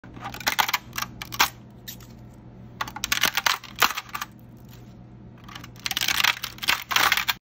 SMART (Save Material & Reduce Time) support removal! ✂
We manually remove as much as possible and then let it soak in our ORYX SCA3600 tank to remove the rest.